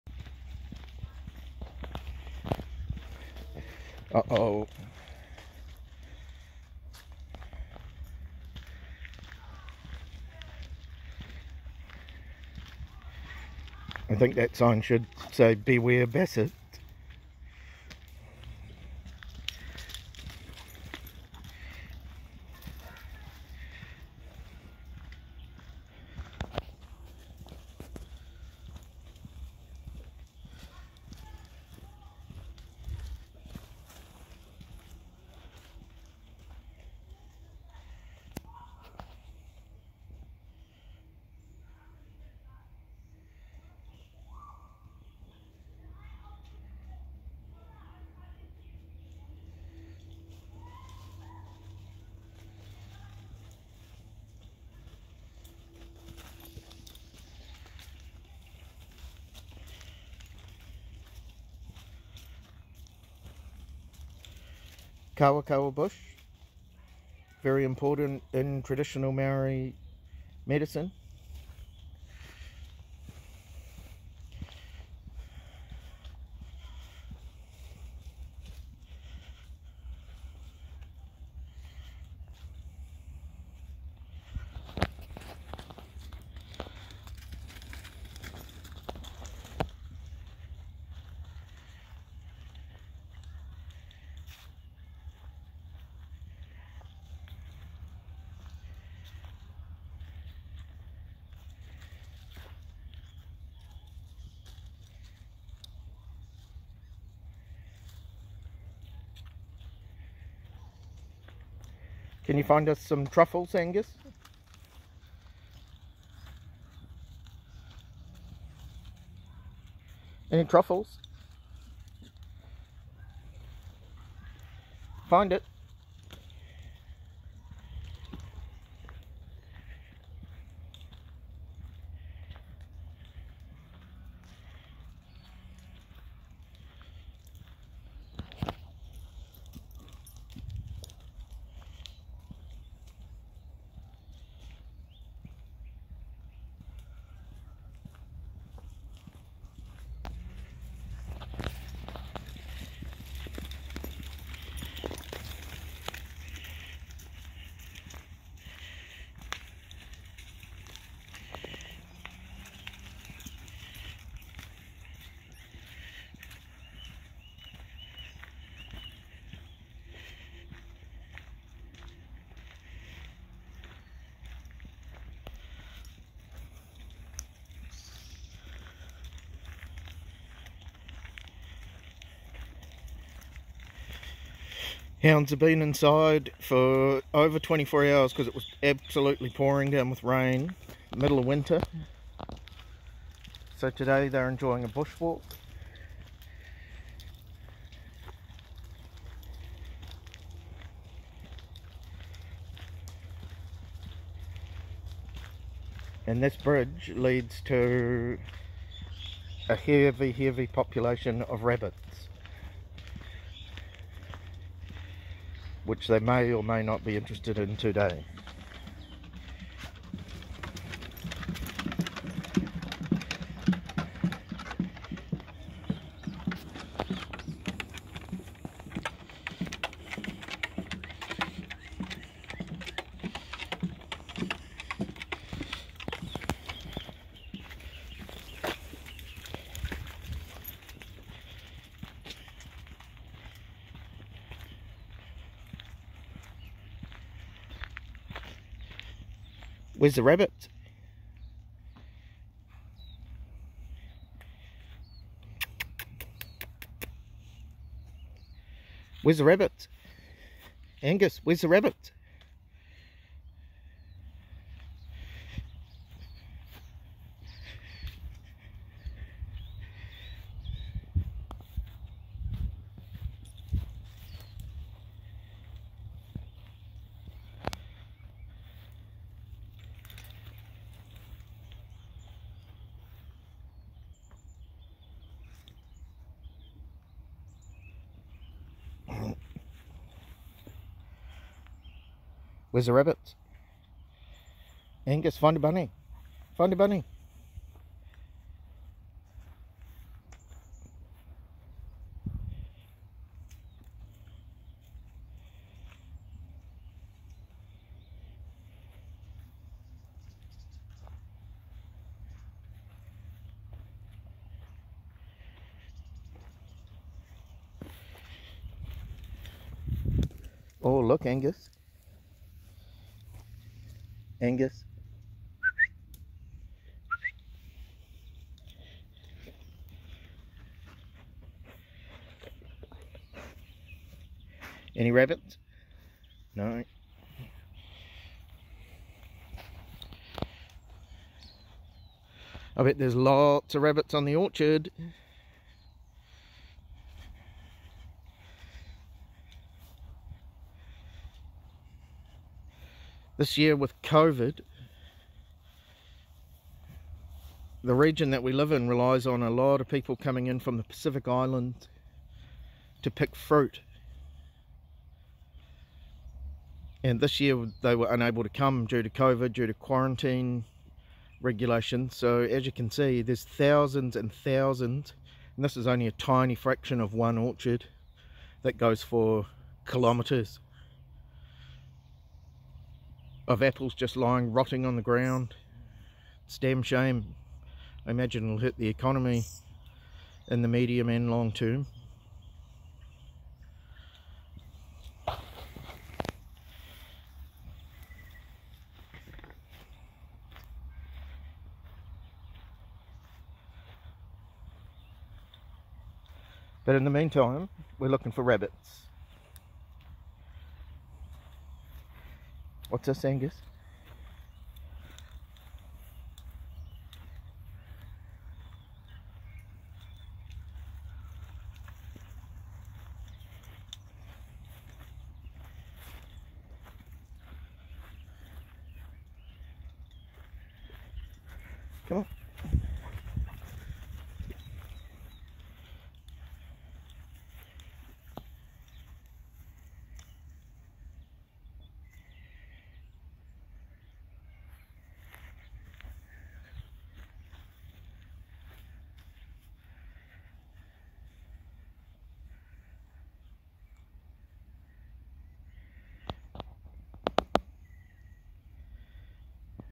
Basset Hounds out & about after being stuck inside
Milli and Angus have been stuck inside during some bad weather.  Today they go for a well deserved walk